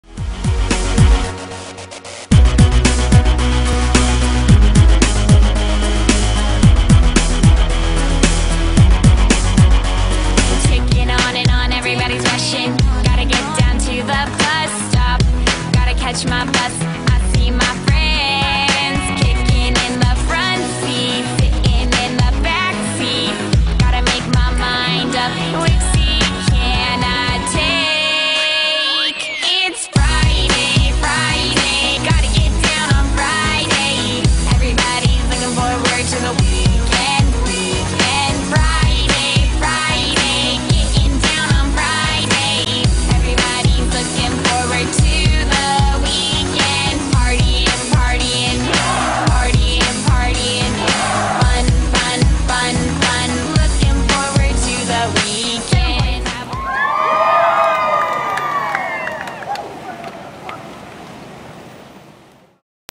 SNU NYC Music Video
SNU Crew helped throw together a quick music video to the first 30 people that showed up at a specified location at NYC.
Keep your eyes for open for more 30 second videos from Louisville!